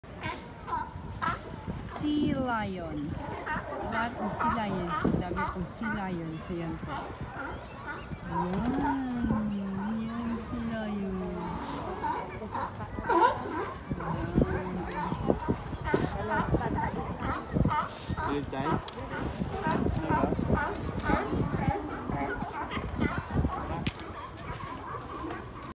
Sea Lions at Pier 39 San francisco California@lynettecartagena54
Lynette’s tv vlog family watching andami g Sea Lions# San Francisco California#Pier 39